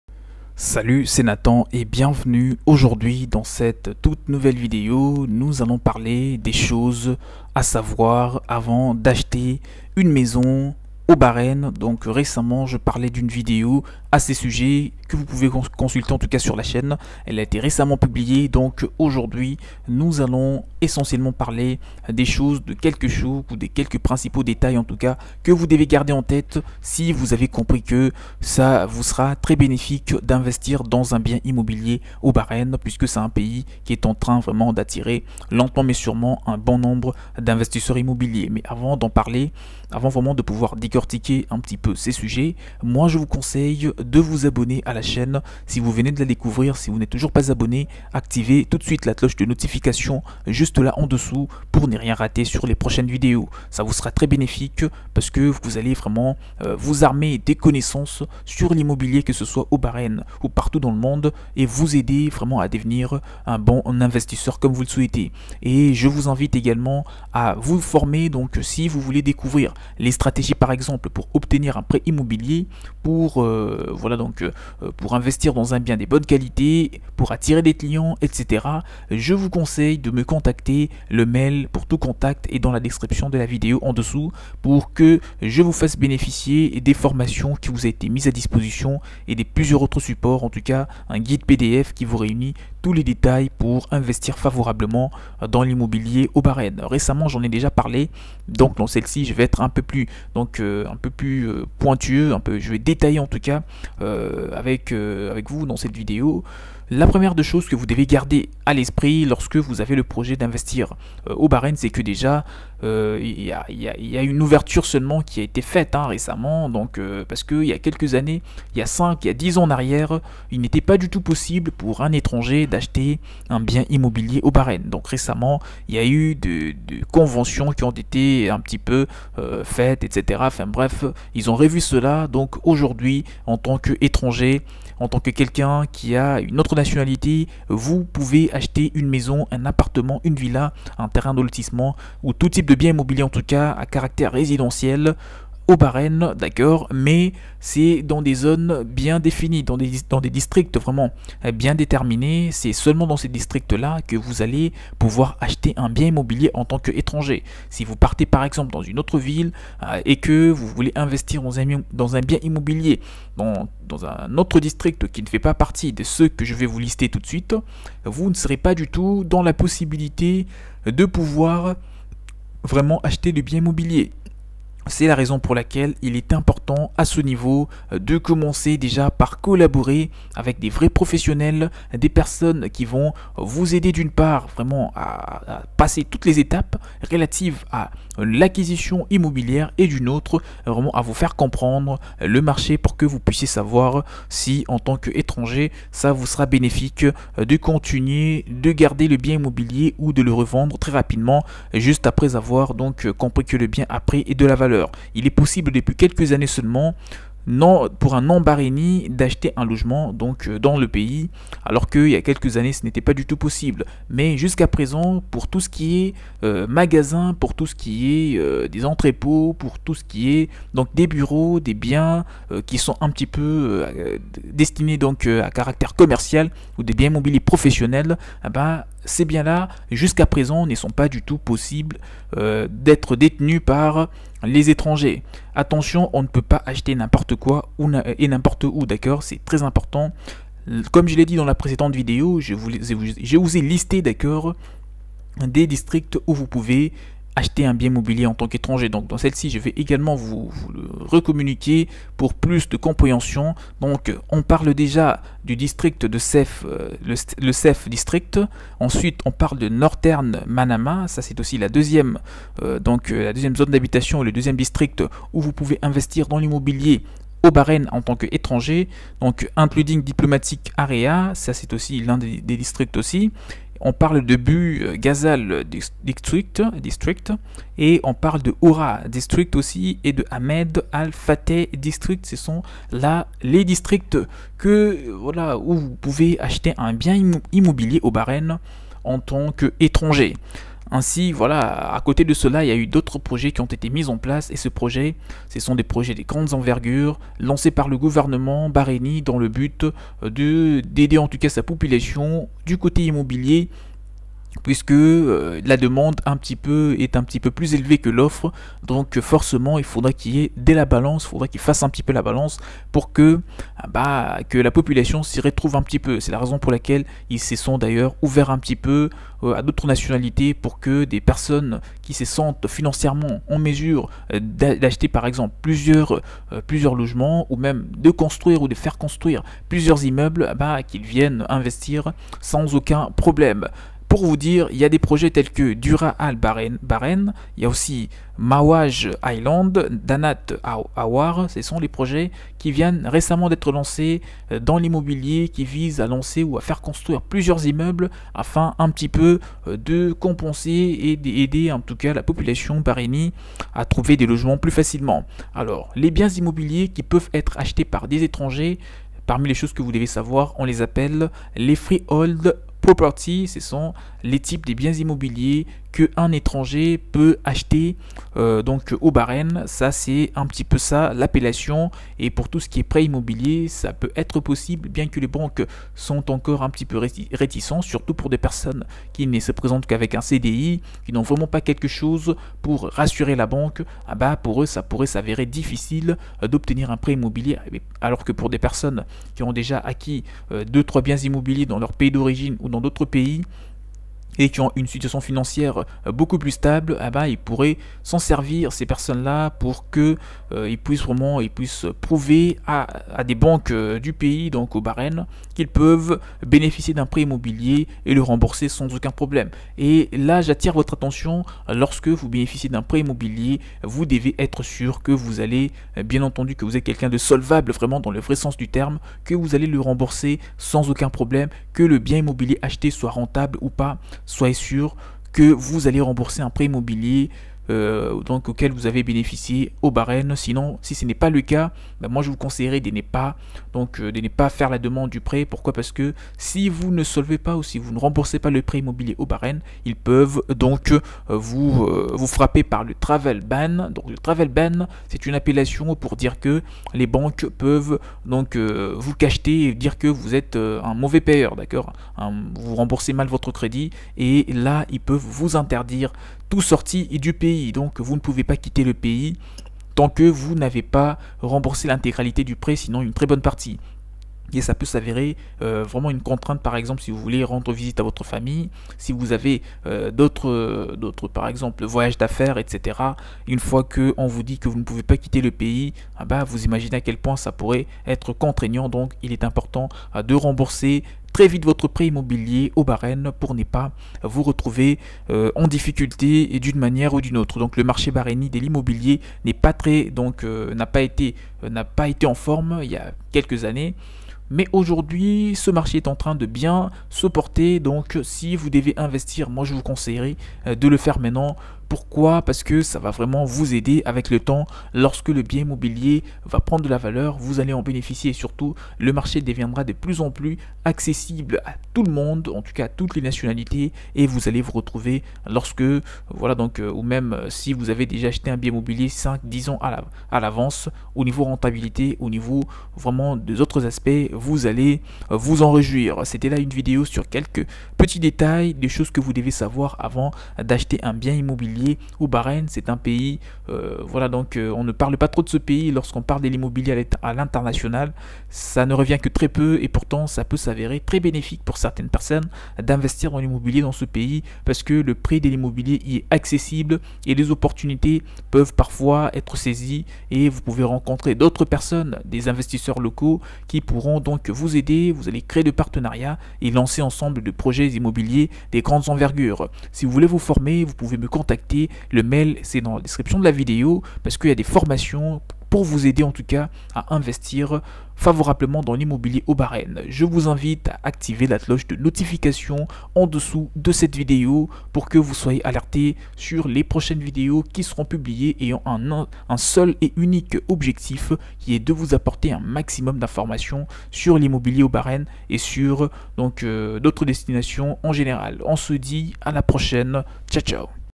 Acheter une maison au Bahreïn - Tout ce qu'il faut savoir
L'immobilier au Bahreïn est très rentable et le marché est à son pic actuellement.Donc,dans cette vidéo,je vous explique comment acheter son premier bien immobilier au Bahreïn en tant qu'étranger et vous constituer progressivement un grand patrimoine.

Site : Gaindereve.com